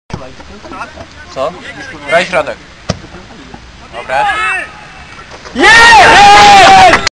2008.11.12 Podlasie II Sokołów P. - Sęp Żelechów: bramka Michała R. Skwarka na 0:1
Sęp przeważał w pierwszej połowie meczu czego udokumentowaniem była bramka Michała Roberta Skwarka w 35. min, dająca żelechowiakom prowadzenie 0:1.